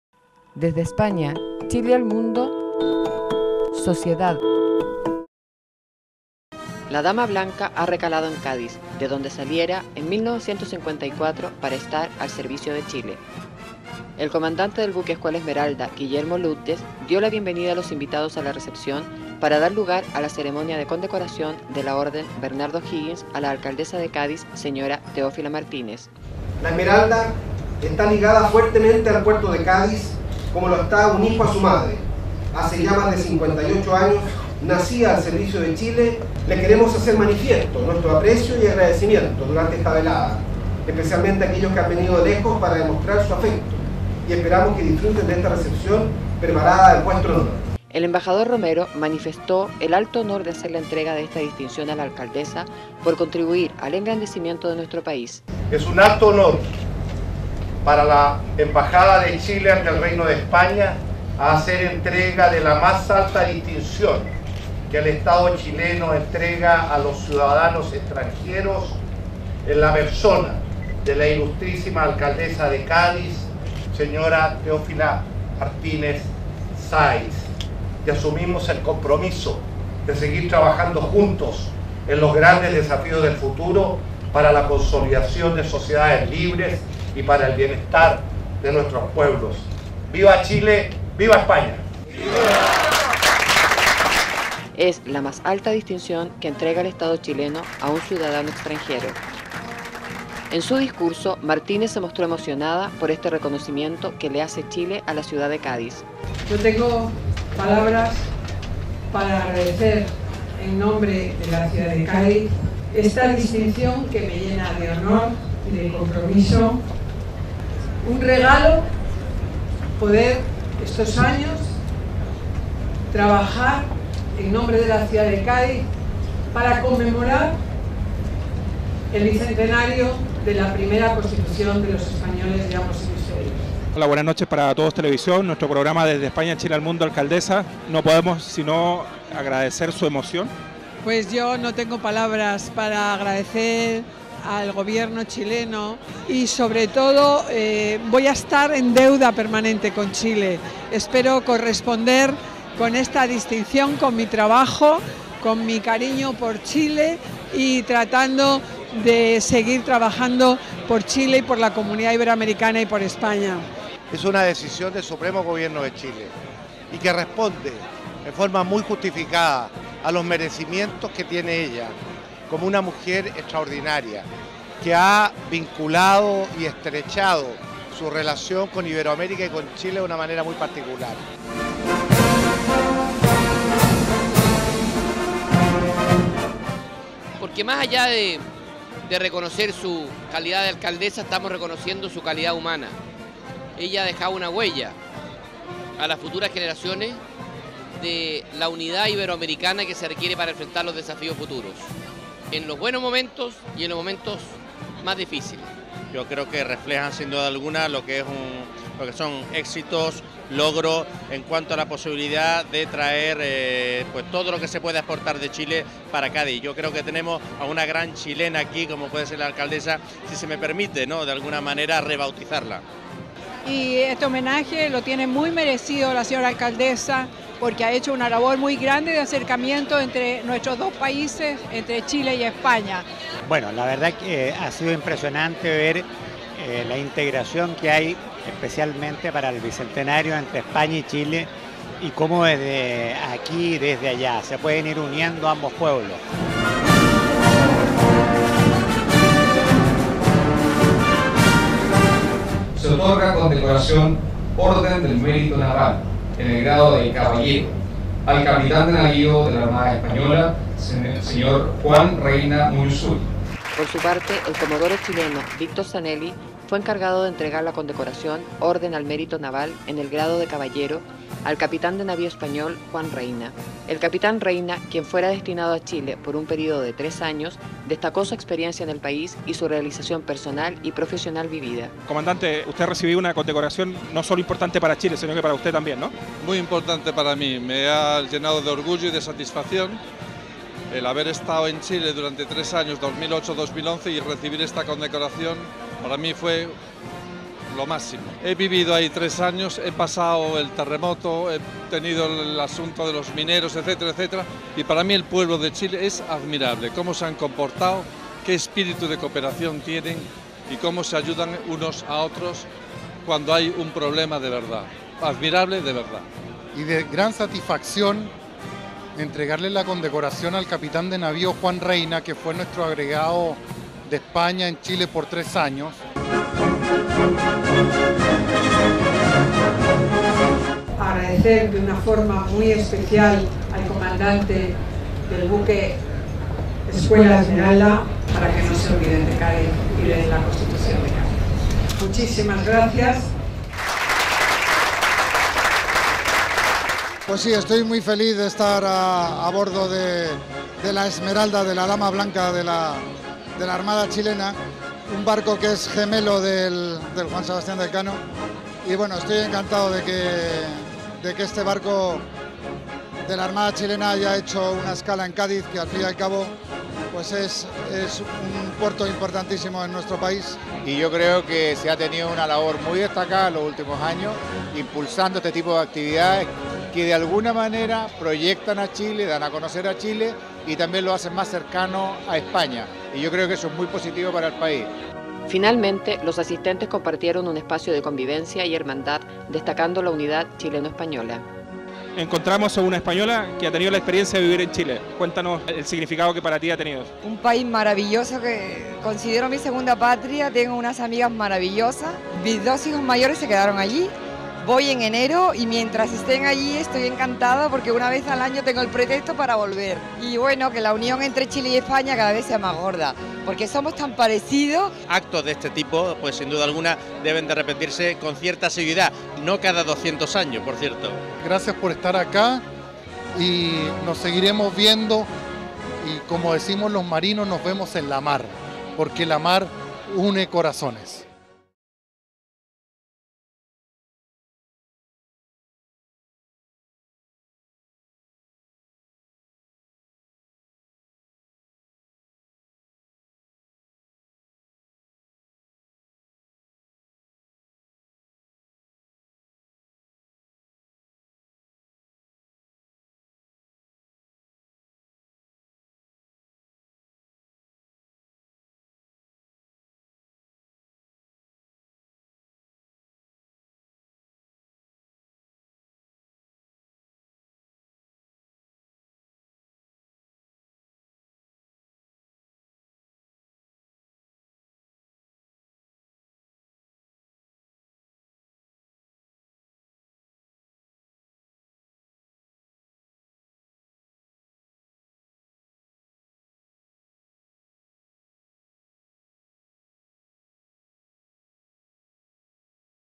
C H I L E AL M U N D O - Buque Esmeralda
Buque Escuela Esmeralda, recalando en CÁDIZ - ESPAÑA. Después de 4 años de ausencia en el puerto Gaditano. La recepción  en la Dama Blanca estuvo a cargo del Comandante de la Armada Chilena Guillermo Lüttges. Siguiendo con las condecoraciones de la Alcaldesa de Cádiz y al Capitán de Navío Español Juan Reyna.